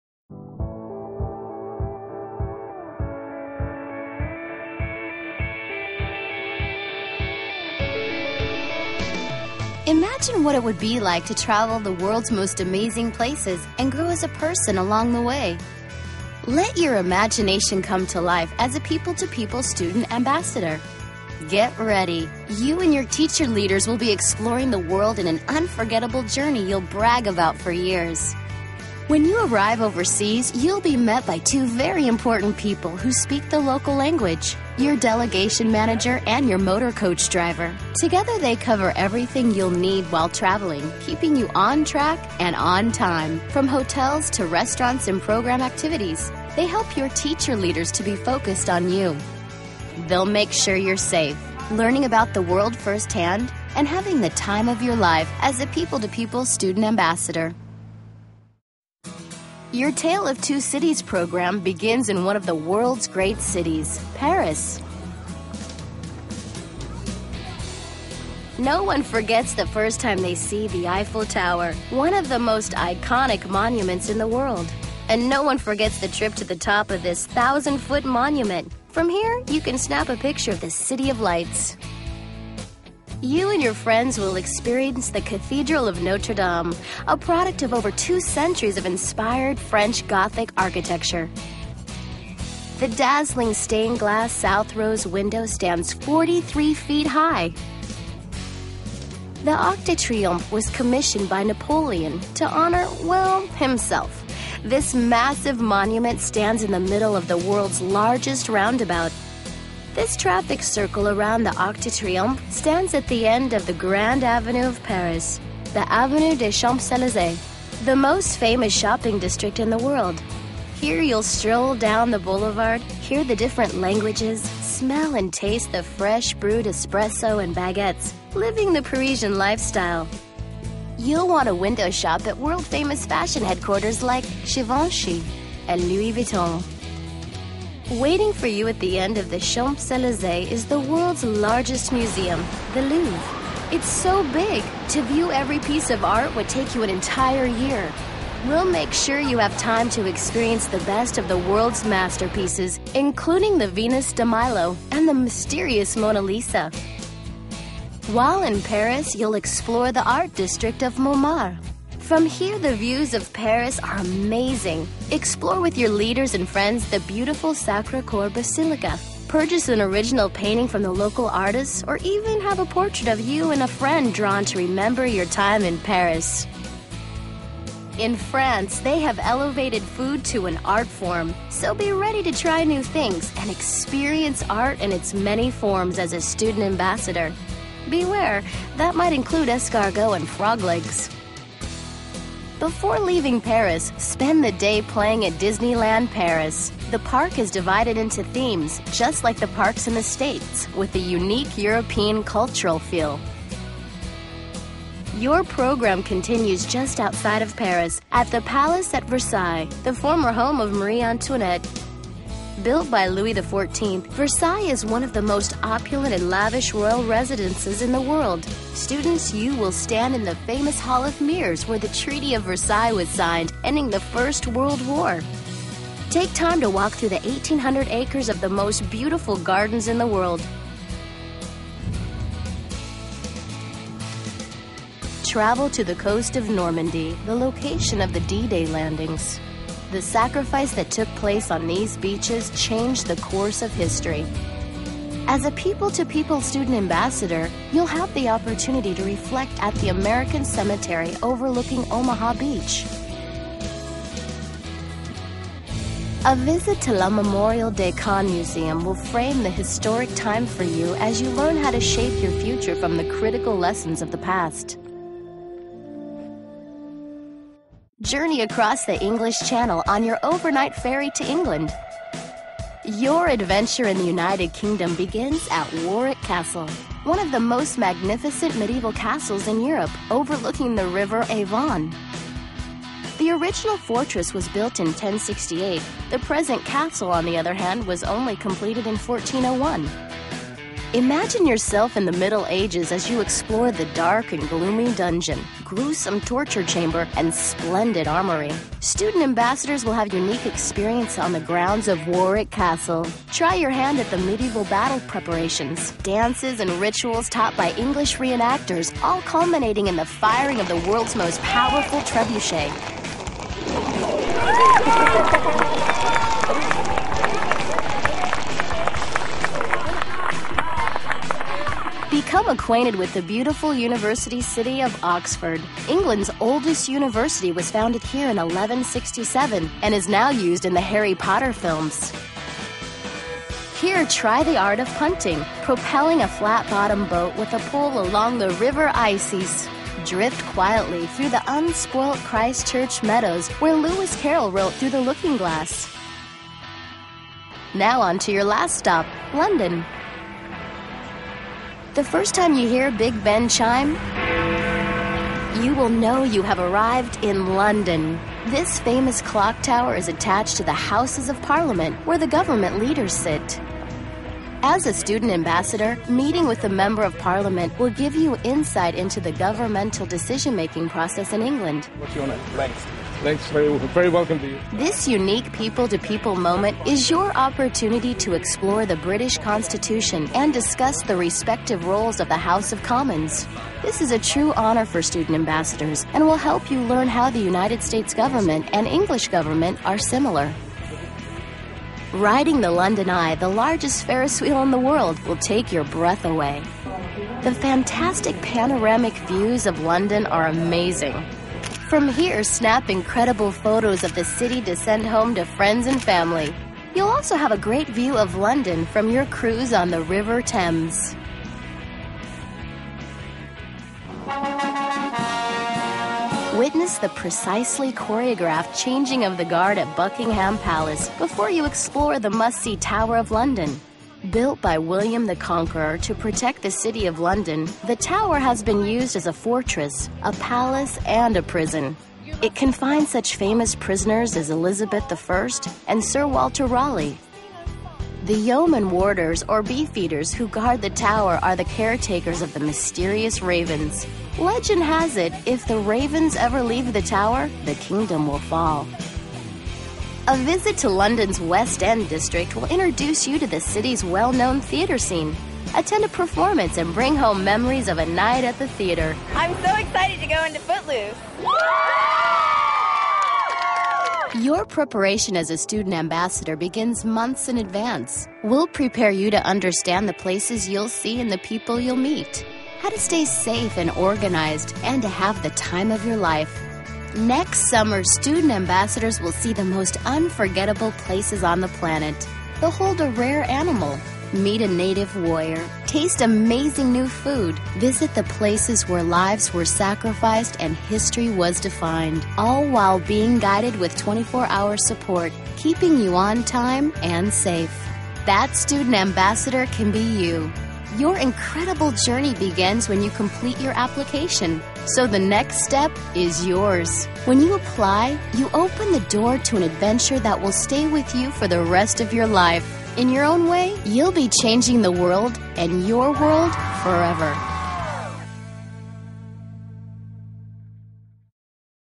People to People Ambassador Programs - Tale Of Two Cities (Grade Level: 5th - 6th)
Destination:
London to Paris (England & France)

Activities:
Briefing with Member of Parliament, ride the London Eye, Changing of the Guard at Buckingham Palace,  Exclusive access experience at Warwick Castle, Stonehenge, Beaches of Normandy, Eiffel Tower, Disneyland Paris